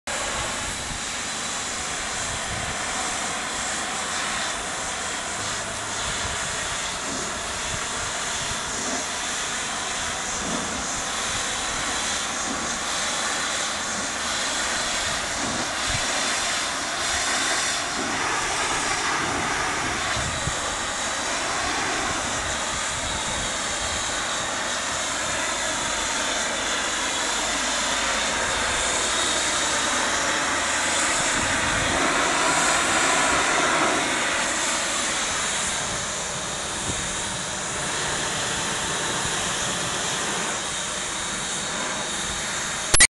MikesMobileShine - Modular School Pressure Washing
These modules are assembled and Shipped in from BC.  They come in filthy from the long wet drive through the mountains.